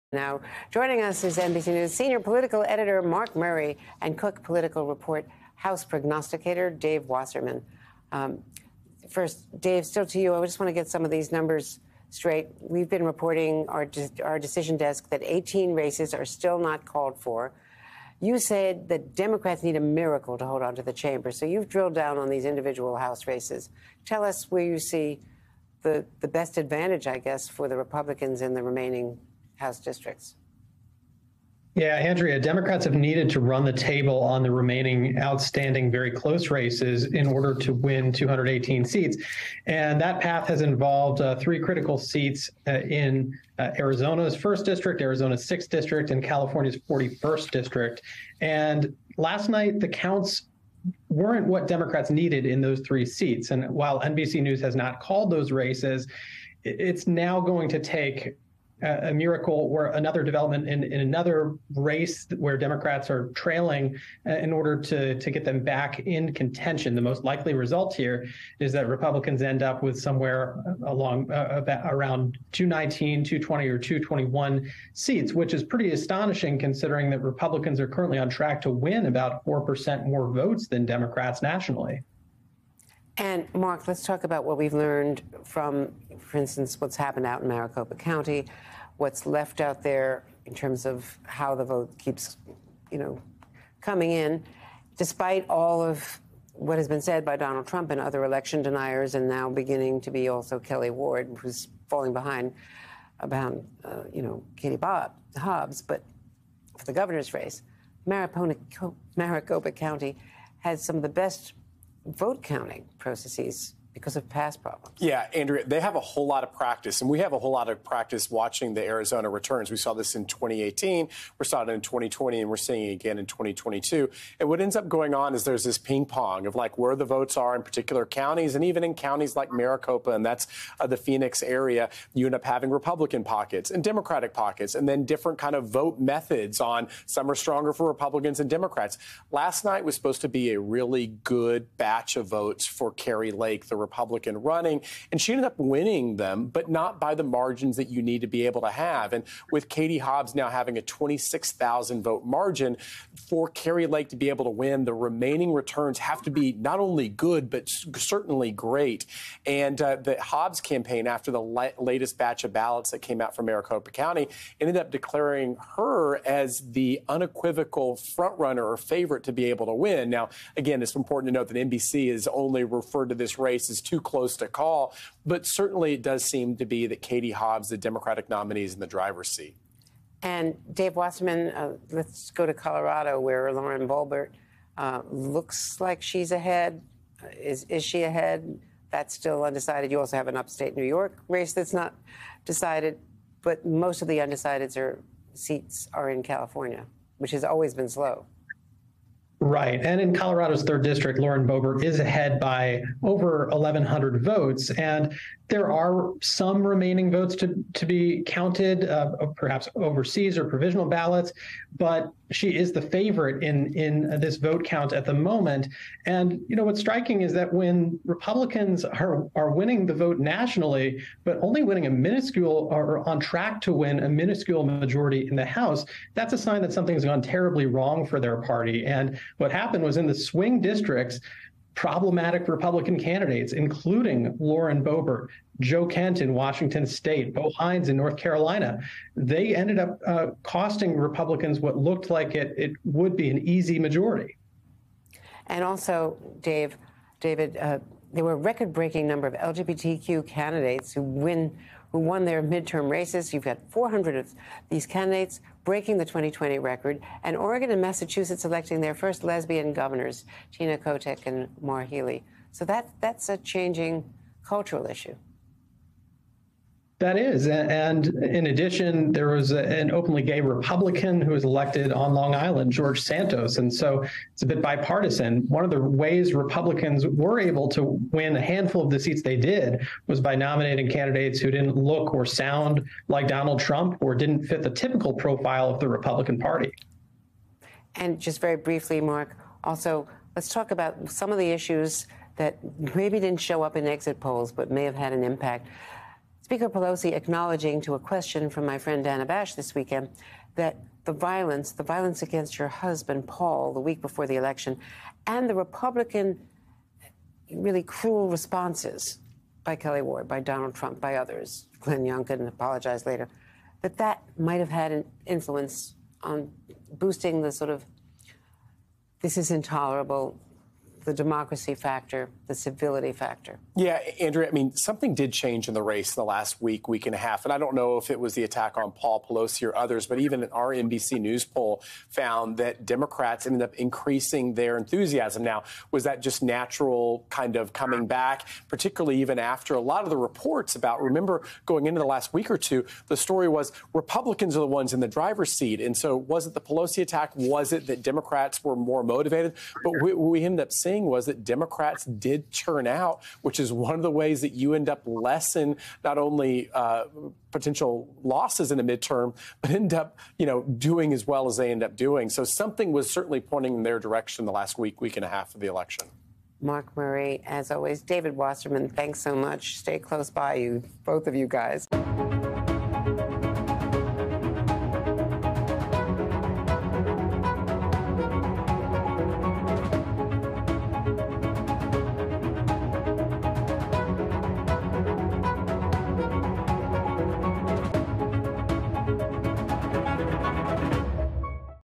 Wasserman: ‘It’s Now Going To Take A Miracle’ To Get Dems ‘Back In Contention’
NBC News Senior Political Editor Mark Murray and Cook Political Report Senior House Editor David Wasserman join Andrea Mitchell as the count for House control continues to unfold. “It's now going to take a miracle, or another development in another race where Democrats are trailing, in order to get them back in contention,” says Wasserman. “The most likely result here is that Republicans end up with somewhere around 219, 220, or 221 seats.”

Voters casted their ballots and results of the 2022 midterm elections are still trickling in. MSNBC is following the most important races across the United States

David Wasserman: ‘It’s Now Going To Take A Miracle’ To Get Democrats ‘Back In Contention’